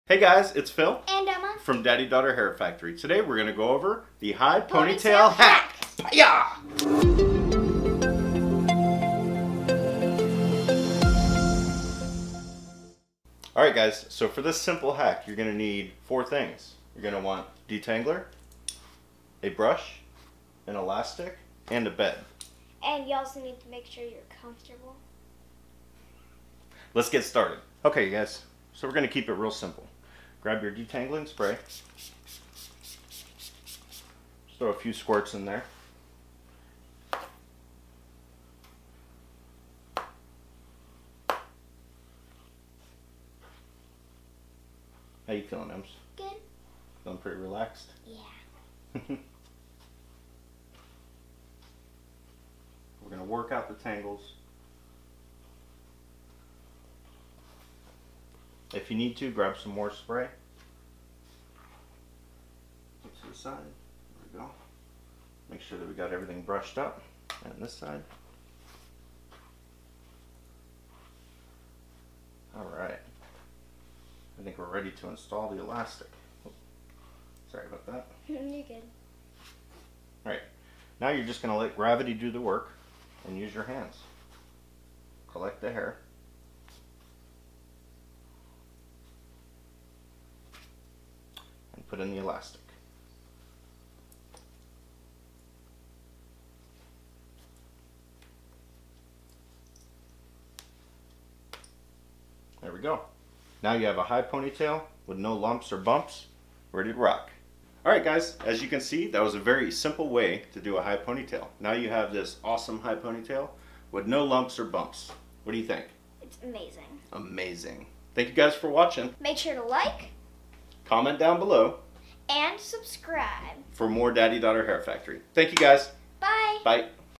High Ponytail Hack by Phil & Emma from Daddy Daughter Hair Factory
This is super simple and probably beneath most of you who struggle with hair, but if you have a hard time doing high ponytails without all of the lumps, try this!

-Phil & Emma